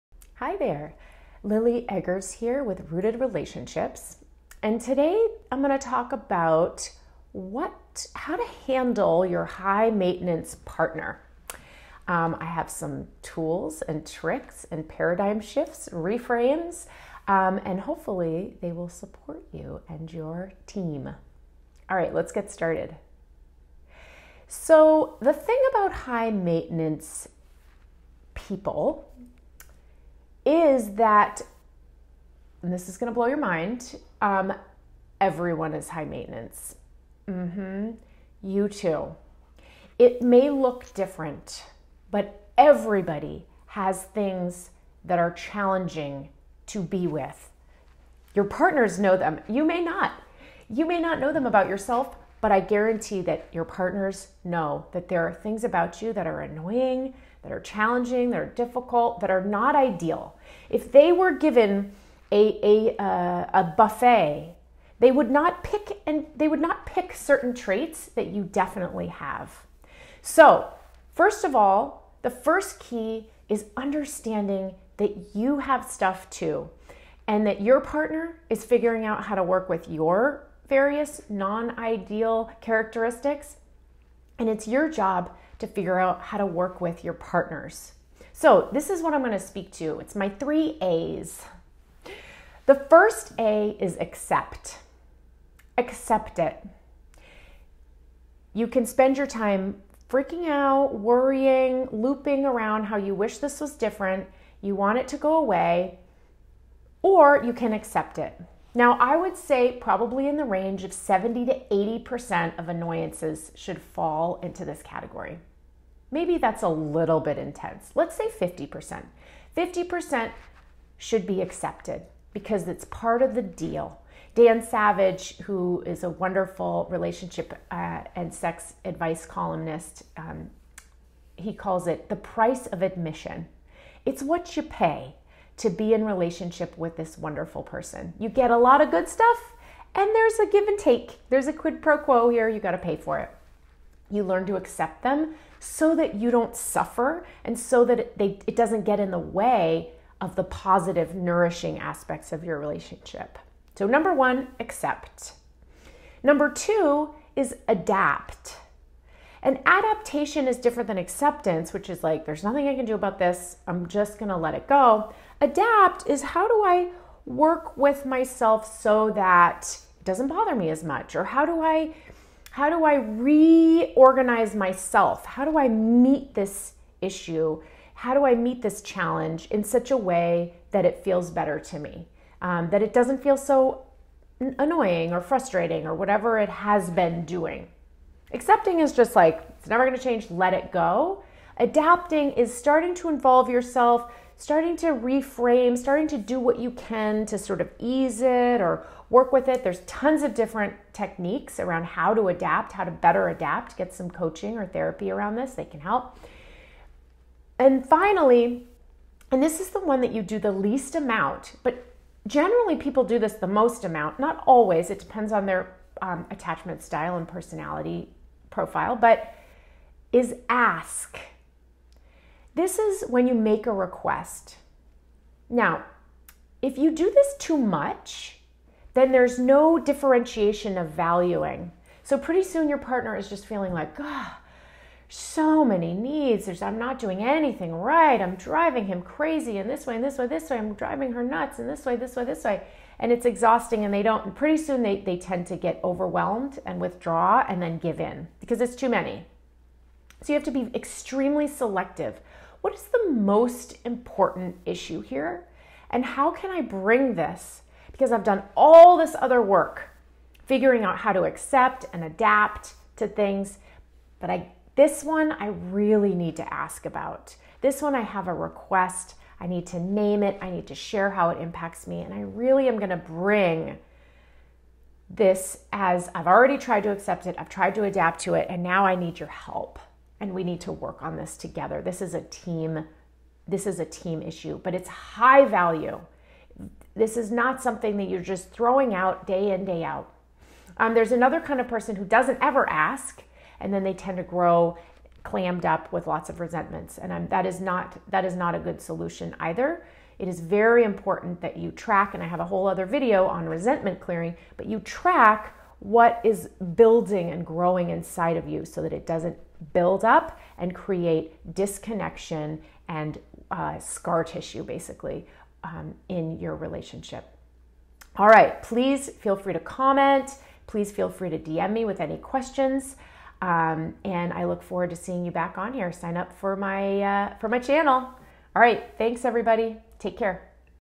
Accept/Adapt/Ask
What to do with those annoying qualities of your partner's?